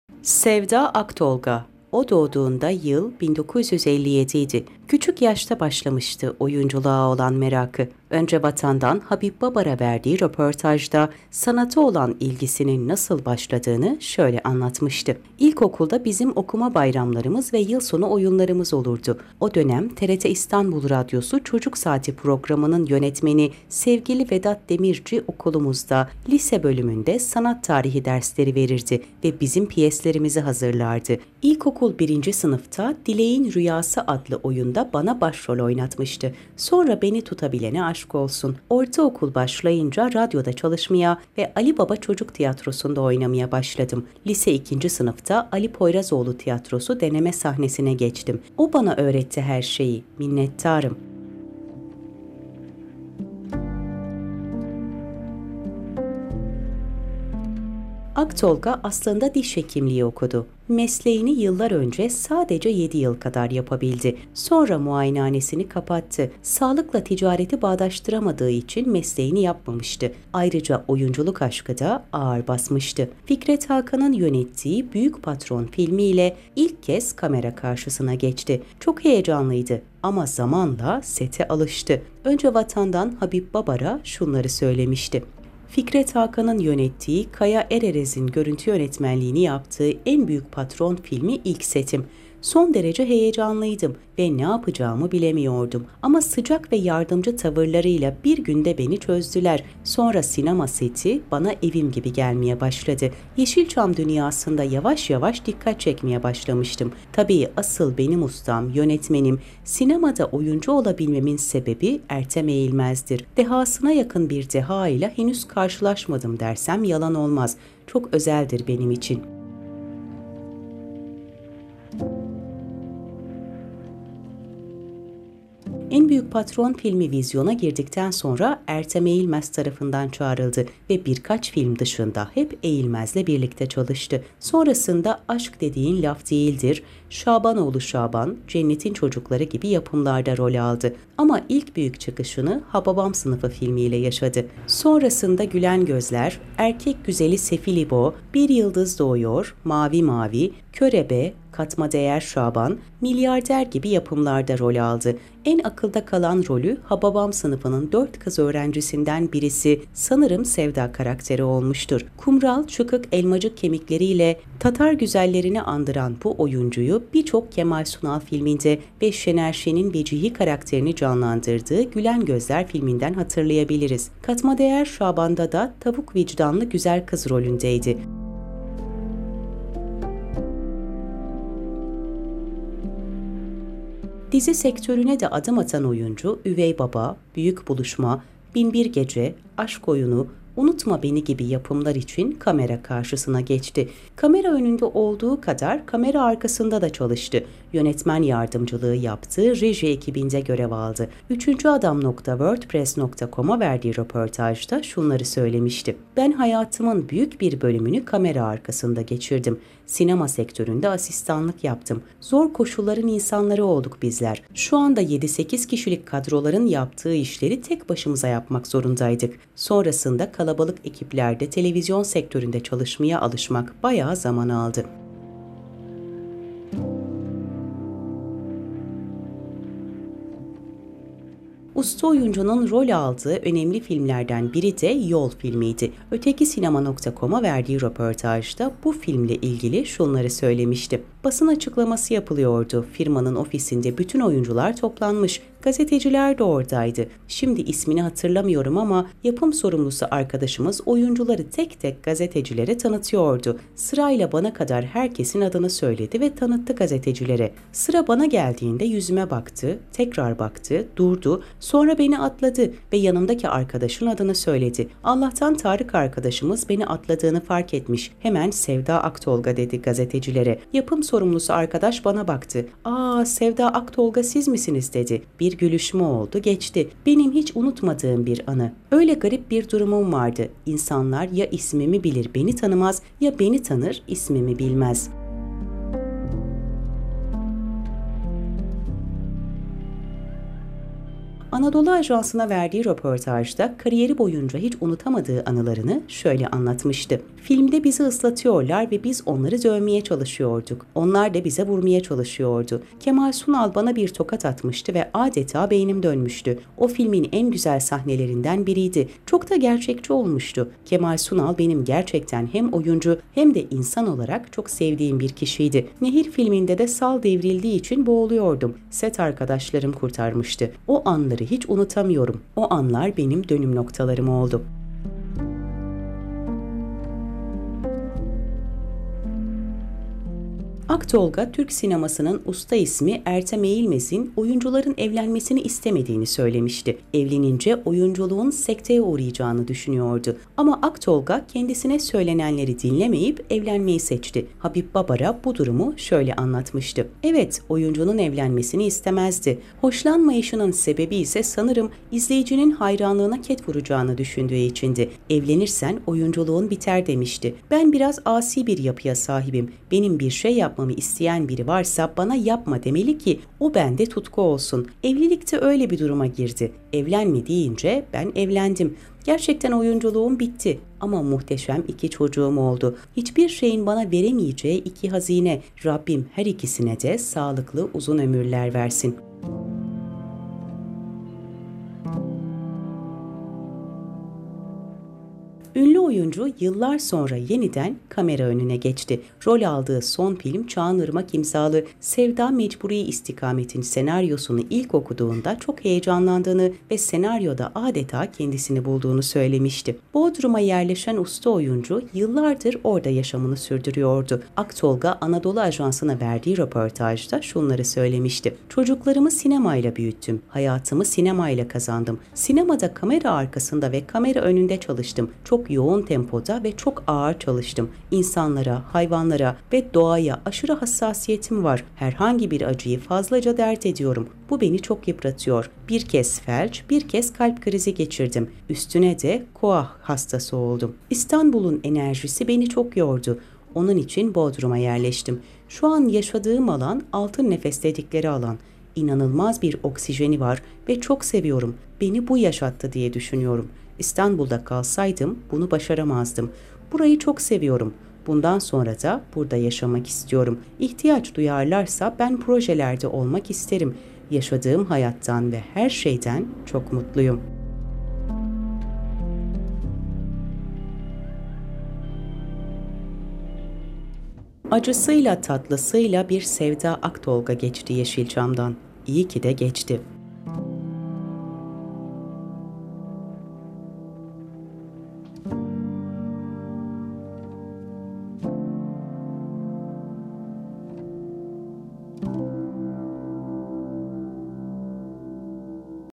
SEVDA AKTOLGA | Yeniden Ekranlara Dönen Hababam Sınıfının Yılıdızı Sevda Aktolga Kimdir?
SEVDA AKTOLGA O DOĞDUĞUNDA YIL 1957YDİ. KÜÇÜK YAŞTA BAŞLAMIŞTI OYUNCULUĞA OLAN MERAKI.  ÖNCEVATANDAN HABİB BABARA VERDİĞİ RÖPORTAJDA SANATA OLAN İLGİSİNİN NASIL BAŞLADIĞINI ŞÖYLE ANLATMIŞTI: İLKOKULDA BİZİM OKUMA BAYRAMLARIMIZ VE YILSONU OYUNLARIMIZ OLURDU… O DÖNEM TRT İSTANBUL RADYOSU ÇOCUK SAATİ PROGRAMI’NIN YÖNETMENİ SEVGİLİ VEDAT DEMİRCİ OKULUMUZDA, LİSE BÖLÜMÜNDE SANAT TARİHİ DERSLERİ VERİRDİ VE BİZİM PİYESLERİMİZİ HAZIRLARDI. İLK OKUL BİRİNCİ SINIFTA ‘DİLEĞİN RÜYASI’ ADLI OYUNDA BANA BAŞROL OYNATMIŞTI. SONRA BENİ TUTABİLENE AŞK OLSUN… ORTA OKULA BAŞLAYINCA RADYODA ÇALIŞMAYA VE ‘ALİ BABA ÇOCUK TİYATROSU’NDA OYNAMAYA BAŞLADIM. LİSE İKİNCİ SINIFTA ‘ALİ POYRAZOĞLU TİYATROSU DENEME SAHNESİ’NE GEÇTİM. O BANA ÖĞRETTİ HER ŞEYİ MİNNETTARIM..
AKTOLGA ASLINDA DİŞ HEKİMLİĞİ OKUDU. MESLEĞİNİ YILLAR SONCE SADECE 7 YIL KADAR YAPABİLDİ SONRA MUAYENEHANESİNİ KAPATTI. SAĞLIKLA TİCARETİ BAĞDAŞTIRAMADIĞI İÇİN MESLEĞİNİ YAPMAMIŞTI. AYRICA OYUNCULUK AŞKI DA AĞIR BASMIŞTI. FİKRET HAKANIN YÖNETTİĞİ BÜYÜK PATRON FİLMİYLE İLK KEZ KAMERA KARŞISINA GEÇTİ. ÇOK HEYECANLIYDI. AMA ZAMANLA SETE ALIŞTI. ÖNCEVATANDAN HABİB BABARA ŞUNLARI SÖYLEMİŞTİ: FİKRET HAKAN‘IN YÖNETTİĞİ, KAYA EREREZ‘İN GÖRÜNTÜ YÖNETMENLİĞİNİ YAPTIĞI ‘EN BÜYÜK PATRON’ FİLMİ İLK SETİM. SON DERECE HEYECANLIYDIM VE NE YAPACAĞIMI BİLEMİYORDUM. AMA SICAK VE YARDIMCI TAVIRLARIYLA BİR GÜNDE BENİ ÇÖZDÜLER. SONRA SİNEMA SETİ BANA EVİM GİBİ GELMEYE BAŞLADI. YEŞİLÇAM DÜNYASINDA YAVAŞ YAVAŞ DİKKAT ÇEKMEYE BAŞLAMIŞTIM. TABİ  ASIL BENİM USTAM, YÖNETMENİM, SİNEMADA OYUNCU OLABİLMEMİN SEBEBİ ERTEM EĞİLMEZ’DİR. DEHASINA YAKIN BİR DEHA İLE HENÜZ KARŞILAŞMADIM DERSEM YALAN OLMAZ… ÇOK ÖZELDİR BENİM İÇİN…
ENBÜYÜK PATRON FİLMİ VİZYONA GİRDİKTEN SONRA ERTEM EĞİLMEZ TARAFINDAN ÇAĞIRILDI VE BİRKAÇ FİLM DIŞINDA HEP EĞİLMEZ LE BİRLİKTE ÇALIŞTI. SONRASINDA AŞK DEDİĞİN LAF DEİLDİR, ŞABAN OĞLU ŞABAN, CENNETİN ÇOCUKLARI GİBİ YAPIMLARDA ROL ALDI AMA İLK BÜYÜK ÇIKIŞINI HABABAM SINIFI FİLMİYLE YAŞADI. SONRASINDA GÜLEN GÖZLERİ ERKEK GÜZELİ SEFİL BİLO, BİR YILDIZ DOĞUYOR, MAVİ MAVİ, KÖREBE, KATMA DEĞER ŞABAN, MİLYARDER GİBİ YAPIMLARDA ROL ALDI. EN AKILDA KALAN ROLÜ, HABABAM SINIFININ 4 KIZ ÖĞRENCİDEN BİRİSİ SANIRIM SEVDA KARAKTERİ OLMUSTUR. KUMRAL, ÇIKIK ELMACIK KEMİKLERİ İLE TATAR GÜZELLERİNİ ANDIRAN BU  OYUNCUYU BİR ÇOK KEMAL SUNAL FİLMİNDE VE ŞENER ŞEN’İN VECİHİ KARAKTERİNİ CANLANDIRDIĞI GÜLEN GÖZLER FİLMİNDEN HATIRLAYABİLİRİZ.“ “KATMA DEĞER ŞABAN’DA DA TAVUK VİCDANLI GÜZEL KIZ ROLÜNDEYDİ. O FİLMİ İZLEDİKTEN SONRA GERÇEKTEN AŞIK OLMUŞTUM. GÜLÜŞÜ GERÇEKTEN DE ÇOK HOŞTU…”
DİZİ SEKTÖRÜNE DE ADIM ATAN OYUNCU ÜVEY BABA, BÜYÜK BULUŞMA, BİNBİR GECE, AŞK OYUNU, UNUTMA BENİ GİBİ YAPIMLAR İÇİN KAMERA KARŞISINA GEÇTİ. KAMERA ÖNÜNDE OLDUĞU KADAR KAMERA ARKASINDA DA ÇALIŞTI YÖNETMEN YARDIMCILIĞI YAPTI REJİ EKİBİNDE GÖREV ALDI. ÜCÜNCUADAM.WORDPRESS.COMA VERDİĞİ RÖPORTAJDA ŞUNLARI SÖYLEMİŞTİ: BEN HAYATIMIN BÜYÜK BİR BÖLÜMÜNÜ KAMERA ARKASINDA GEÇİRDİM. SİNEMA SEKTÖRÜNDE ASİSTANLIK YAPTIM. ZOR KOŞULLARIN İNSANLARI OLDUK BİZLER… ŞU ANDA 7-8 KİŞİLİK KADROLARIN YAPTIĞI İŞLERİ, TEK BAŞIMIZA YAPMAK ZORUNDAYDIK. SONRASINDA KALABALIK EKİPLERDE TELEVİZYON SEKTÖRÜNDE ÇALIŞMAYA ALIŞMAK BAYA ZAMAN ALDI…
USTA OYUNCU’NUN ROL ALDIĞI ÖNEMLİ FİLMLERDEN BİRİ DE YOL FİLMİYDİ.